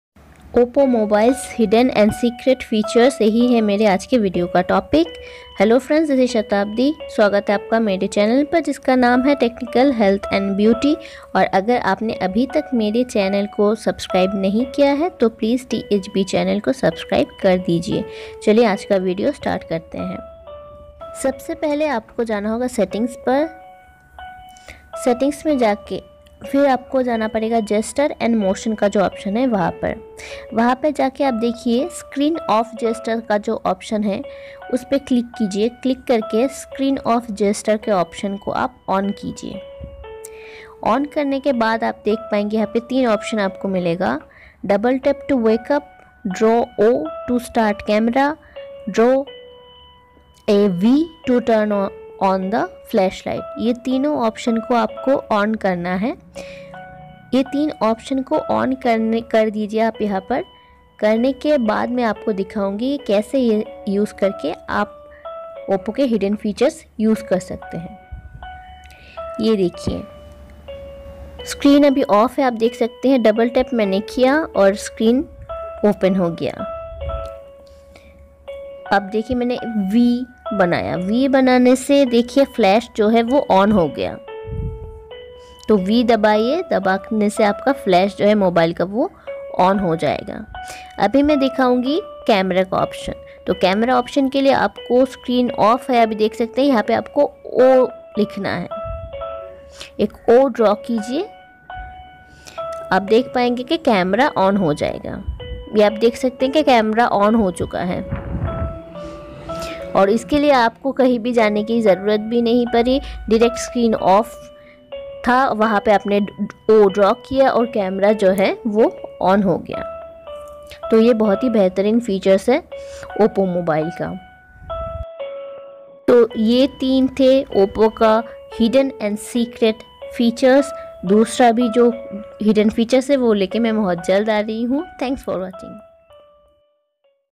OPPO Mobiles Hidden & Secret Features in Hindi | Oppo Mobiles Top Features- Must Know | Oppo a37
OPPO Mobiles Hidden & Secret Features in Hindi | Oppo Mobiles Top  Features- Must Know. Agar OPPO mobile liya hai to zarur dakhe ye video.

Technical, Health, Beauty and Entertainment related videos are uploaded here.

Share this videos with your youtube friends.